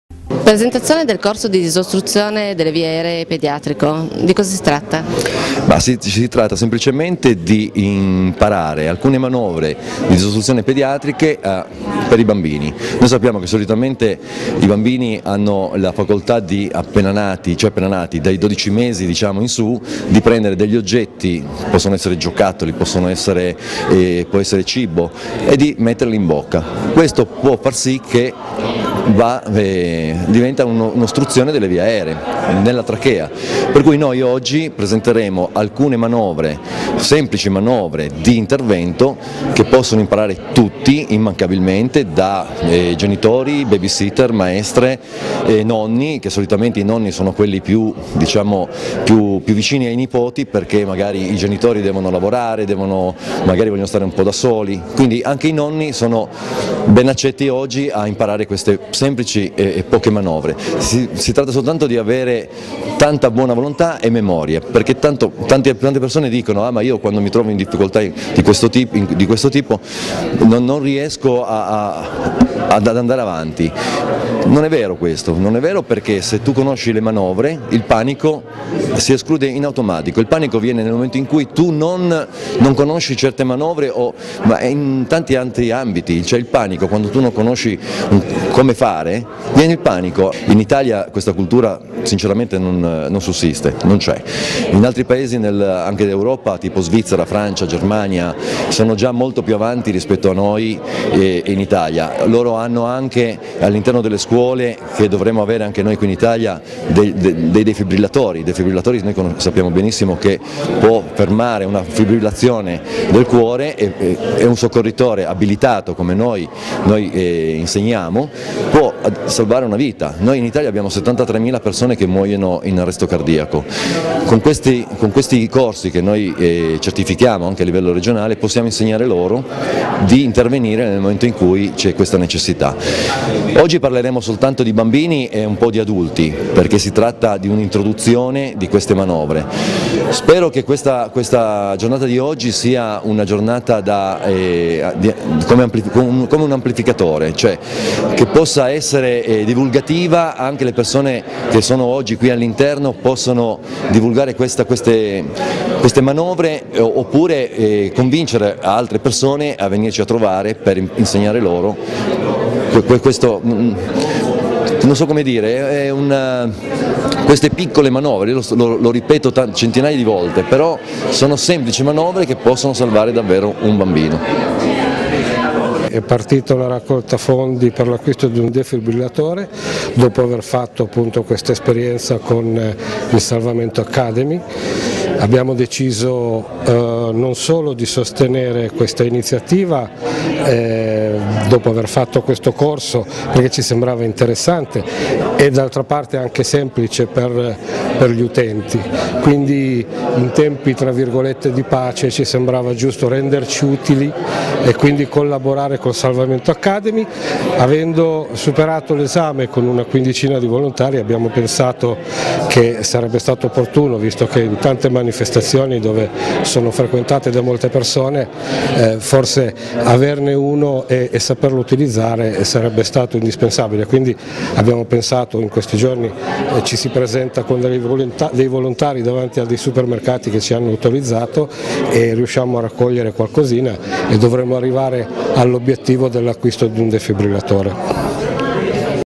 A Valperga un incontro sulla disostruzione pediatrica
Ogni anno in Italia 50 bambini perdono la vita per soffocamento da corpo estraneo, cioè un piccolo oggetto o semplicemente il cibo ingerito. Un dato già di per sé drammatico, ma ancora più grave se si pensa che basterebbero alcune semplici nozioni di disostruzione delle vie aeree per salvar loro la vita. Così sabato a Valperga si è tenuta una interessante lezione sule manovre di disostruzione pediatrica.